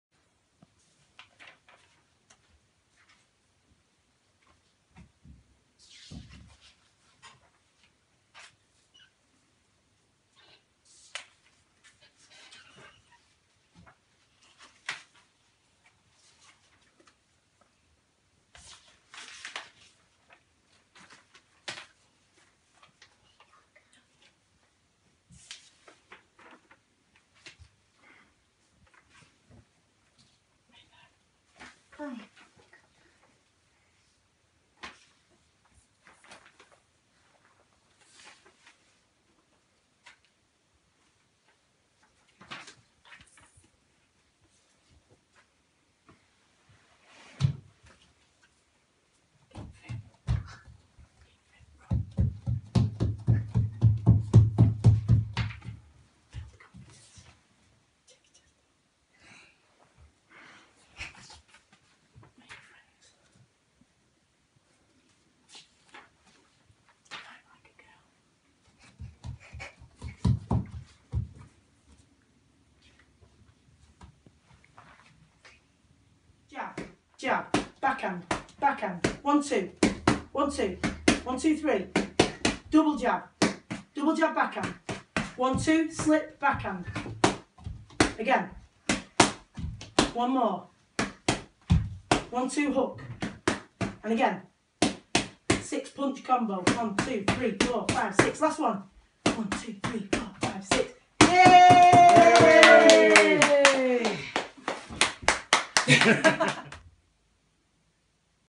England boxer Stacey Copeland and niece Ruby explain the benefits of boxing
To celebrate International Women's Day 2016, England boxer Stacey Copeland and niece Ruby explain the benefits of boxing, with a short demonstration which proves that boxing is a sport that women of all ages can enjoy!